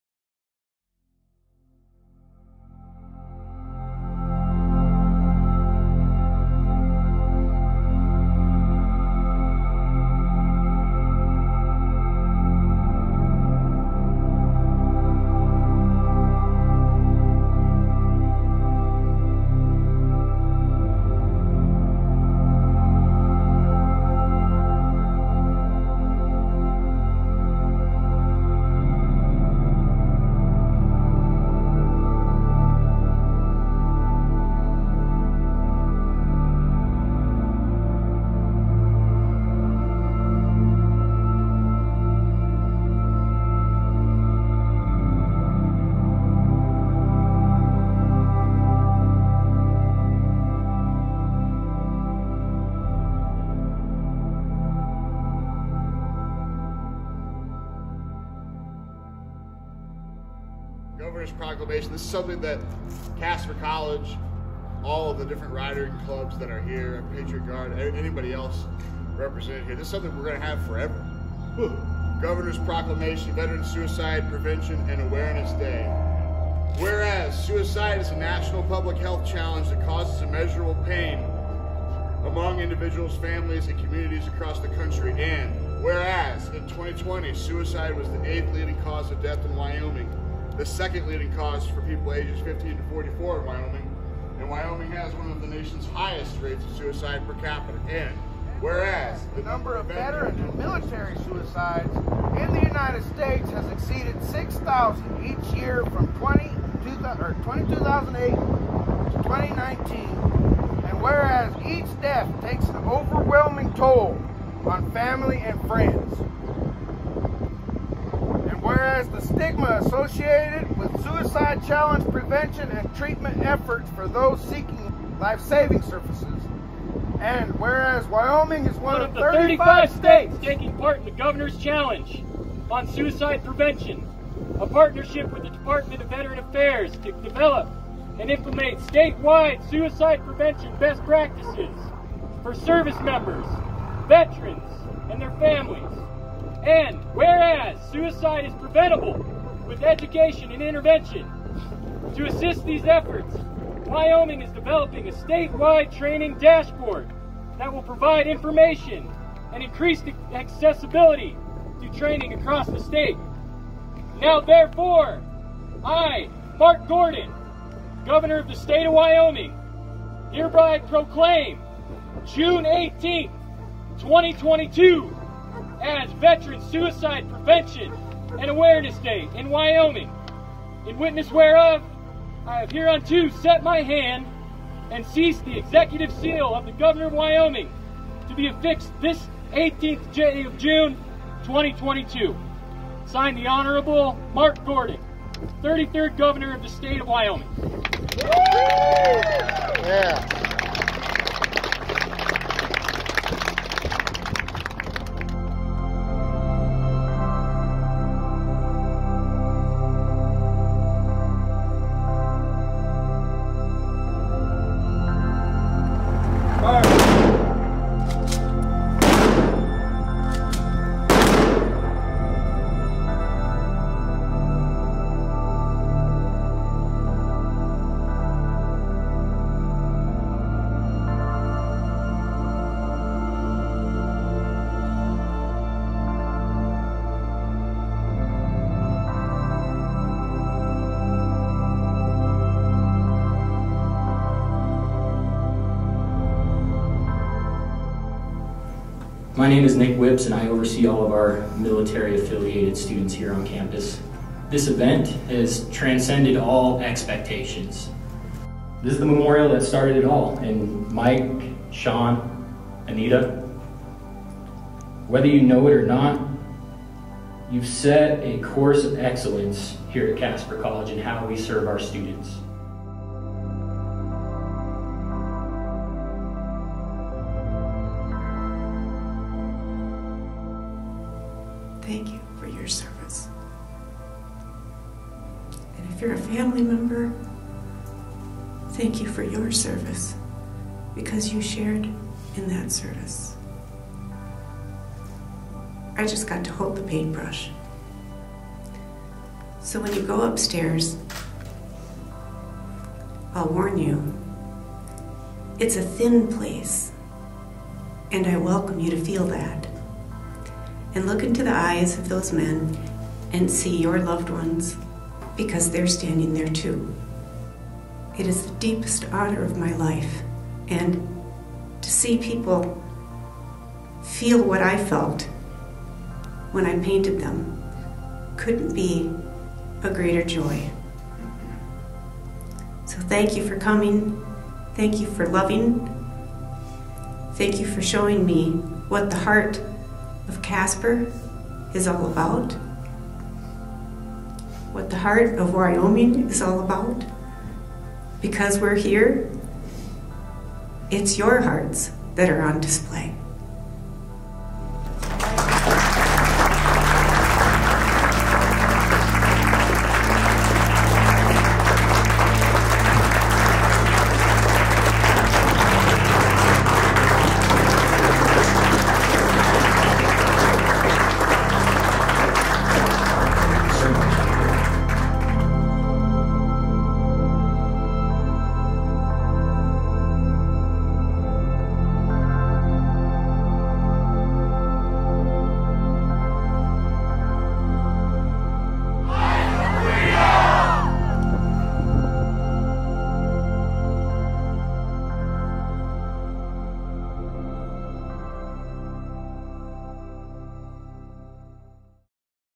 Eyes of Freedom at Casper College - June 2022
This video highlights some of the moments captured during our second visit to Casper College in Casper, Wyoming. Special thanks goes to the entire team at Casper College including Dr. Nick Whipps and Caleb Linsey. Additional proclamation footage courtesy of Dustin Smart.